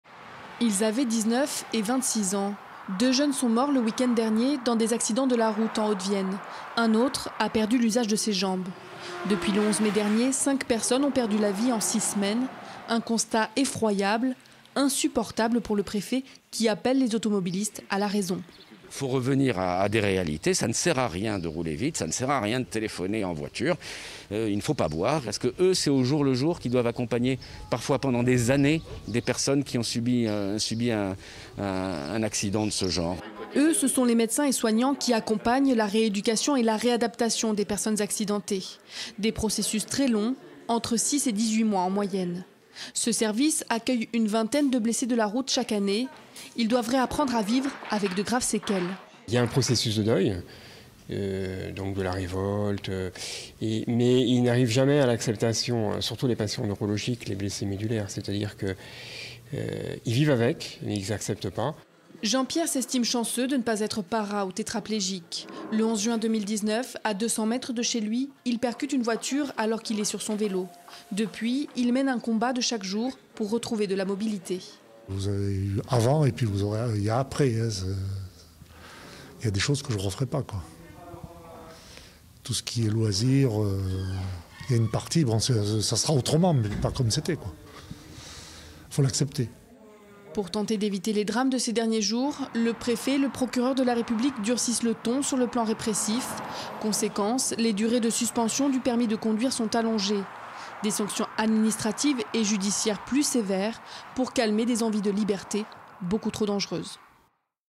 Trop d'accidents en Haute-Vienne
Le préfet de la Haute-Vienne tape du poing sur la table pour dénoncer les comportement dangereux ces derniers jours sur les routes. Plusieurs grands excès de vitesse avec parfois des conséquences tragiques, 2 accidents mortels le weekend dernier en Haute-Vienne.
Le préfet s'est rendu tout à l'heure au service de réadaptation de l’hôpital Jean Rebeyrol à Limoges.

Reportage : Camille Chignac & Samuel Chassaigne